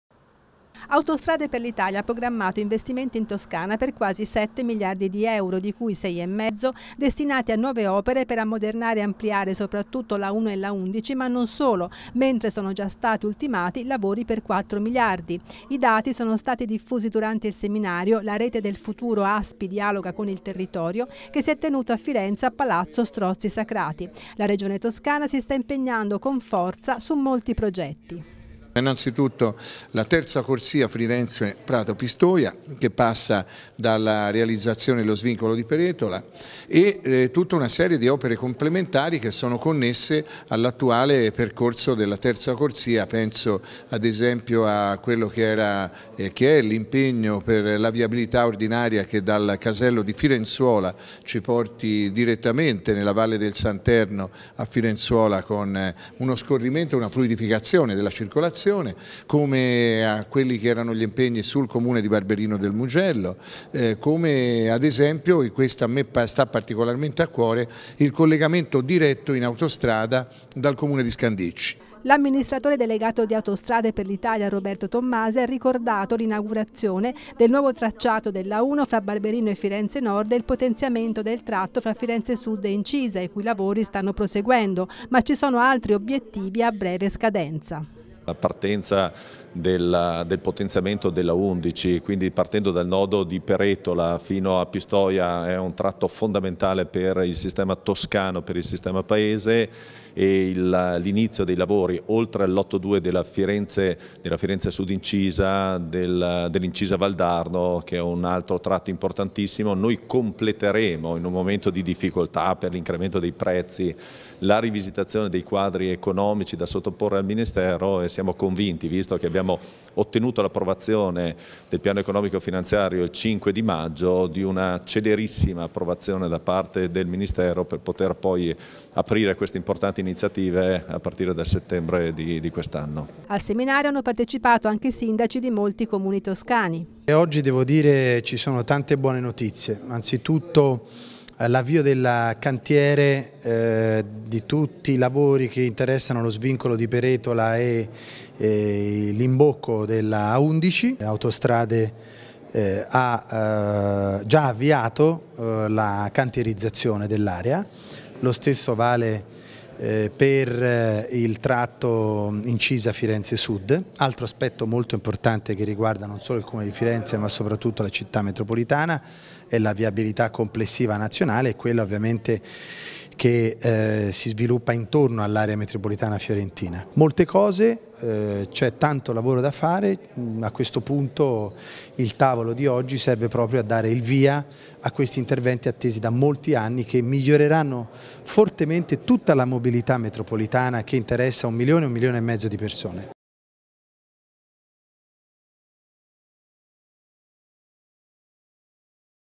Autostrade, 7 miliardi per la Toscana
Il seminario “La rete del futuro", al quale hanno partecipato, fra gli altri, l'amministratore delegato di Autostrade per l'Italia Roberto Tomasi, il sindaco di Firenze Dario Nardella e il presidente della Regione Eugenio Giani, ha fatto il punto sullo stato dei lavori che attualmente interessano la rete infrastrutturale toscana. Fra le novità, al via la realizzazione dello svincolo di Peretola, importante per la città e l’area metropolitana e per l’accesso all’A11, dove nel frattempo cominceranno i lavori della terza corsia e l'apertura dei cantieri dell’A1 da Incisa a Firenze sud, mentre prosegue  la progettazione dello svincolo di Scandicci e i cantieri per la riqualificazione delle carreggiate originarie della A1 tra Barberino e Firenze Nord, dopo l’inaugurazione del nuovo tracciato. Un ulteriore investimento di seicento milioni di euro servirà a portare a termine la realizzazione della terza corsia della A11 tra Firenze e Pistoia, già in fase di cantierizzazione e che in autunno entrerà nel vivo del suo cronoprogramma.